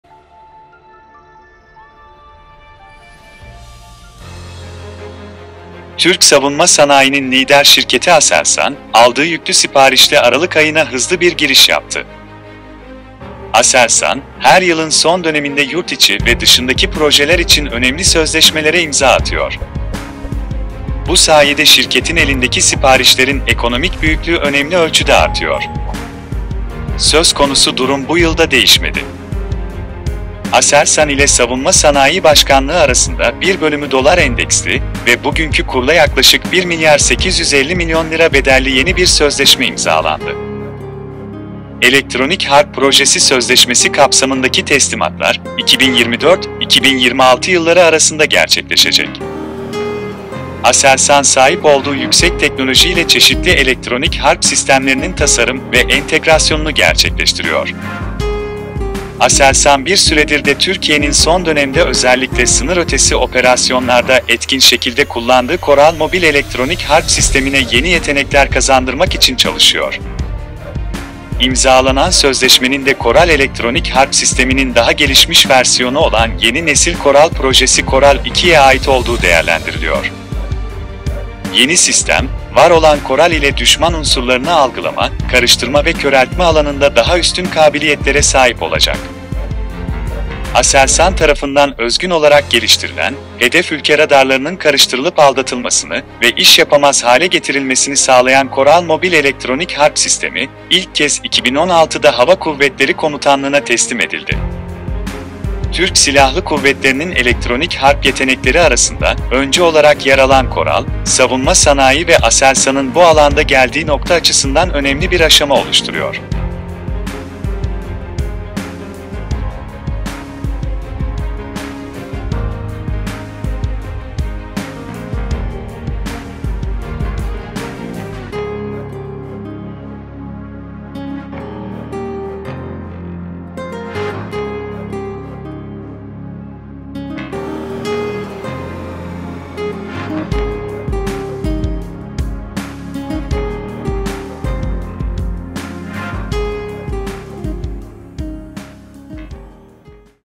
2021 bitiyor, ASELSAN başlıyor - ASELSAN electronic warfare - Savunma Sanayi - Koral 2 - TSK - ASELS
Türk savunma sanayinin lider şirketi ASELSAN, aldığı yüklü siparişle aralık ayına hızlı bir giriş yaptı.

ASELSAN ile Savunma Sanayii Başkanlığı arasında bugünkü kurla yaklaşık 1 milyar 850 milyon lira bedelli yeni bir sözleşme imzalandı. Elektronik harp projesi sözleşmesi kapsamındaki teslimatlar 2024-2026 yılları arasında gerçekleşecek.

İmzalanan sözleşmenin de KORAL Elektronik Harp Sistemi'nin daha gelişmiş versiyonu olan Yeni Nesil KORAL Projesi  KORAL 2’ye ait olduğu değerlendiriliyor. Yeni sistem, var olan Koral ile düşman unsurlarını algılama, karıştırma ve köreltme alanında daha üstün kabiliyetlere sahip olacak.

TSK'nın elektronik harp yetenekleri arasında öncü olarak yer alan Koral, savunma sanayisi ve ASELSAN’ın ( ASELS ) bu alanda geldiği nokta açısından önemli bir aşama oluşturuyor.
ASELSAN electronic warfare

ASELSAN, the leading company of the Turkish defense industry, made a rapid start to December with the large order received.

A new contract was signed between ASELSAN and the Presidency of Defense Industries with a value of approximately 1 billion 850 million liras at today's exchange rates. Deliveries under the electronic warfare project contract will take place between 2024 and 2026.

It is considered that the signed contract belongs to the Next Generation KORAL Project KORAL 2, which is a more advanced version of the KORAL Electronic Warfare System. The new system will have superior capabilities in the field of detecting, mixing and blinding enemy elements with the existing Koral.

Being a pioneer among the TAF's electronic warfare capabilities, Koral is an important step forward for the defense industry and the point ASELSAN has reached in this field.